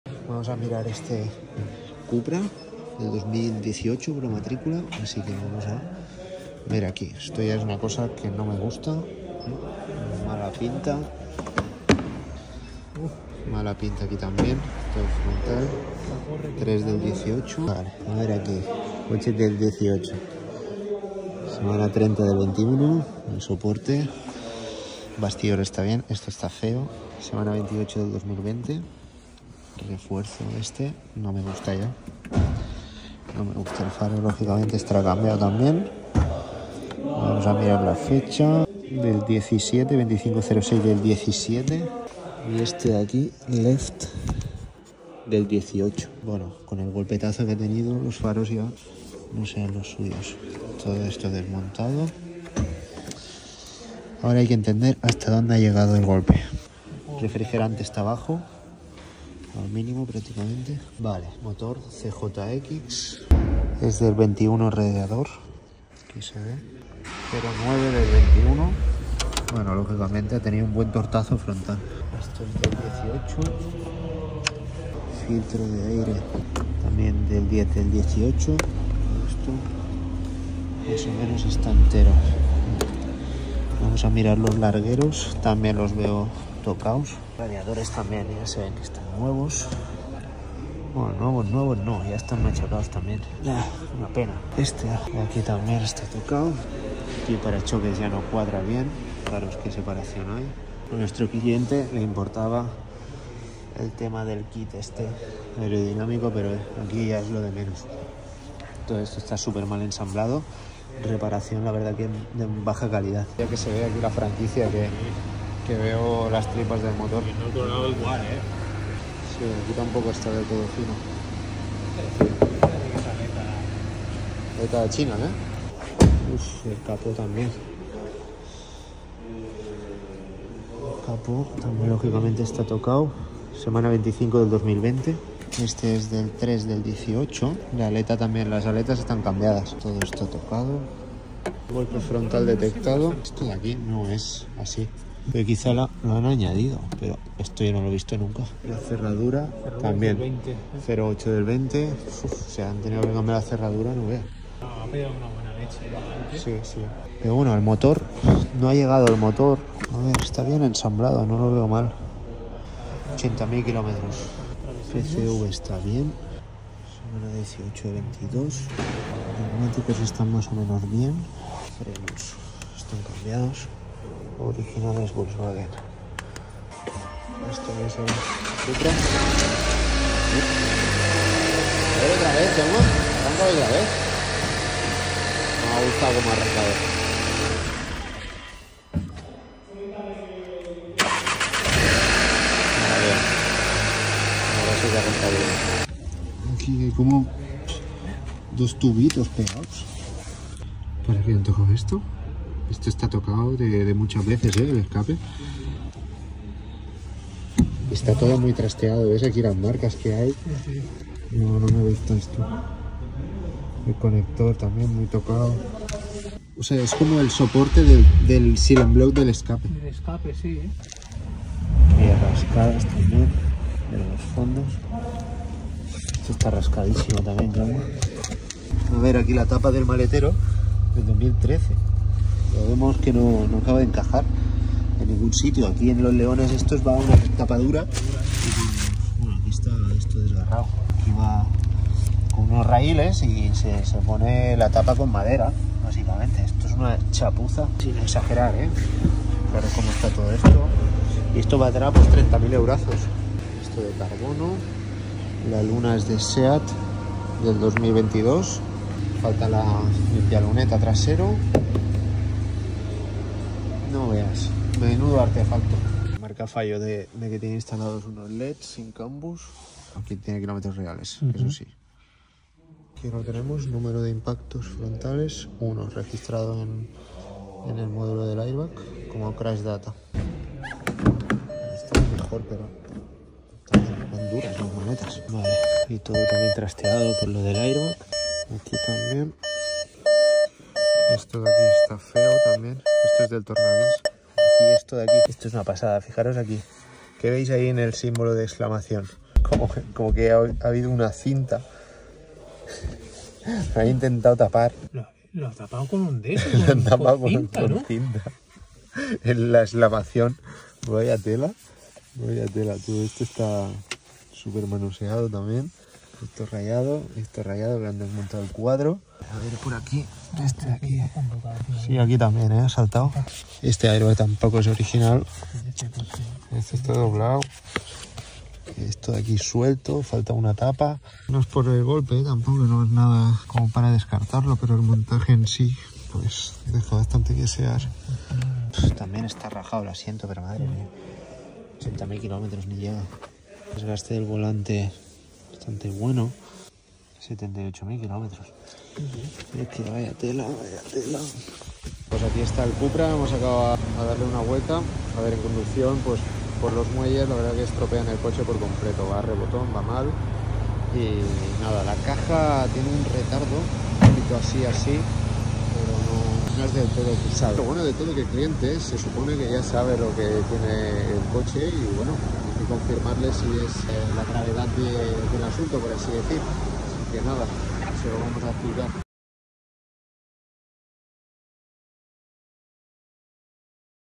¡IMPACTO DELANTERO DETECTADO!🤯 |LEON CUPRA
¿Qué te parece el método de la cinta?🤔
Comenta, ¡te leemos!📥

📍Actualmente revisamos en estas zonas y sus alrededores:
Barcelona | Madrid | Valencia | Málaga | Sevilla | Granada | Murcia | Alicante | Zaragoza | Logroño | Pamplona | Bilbao | A Coruña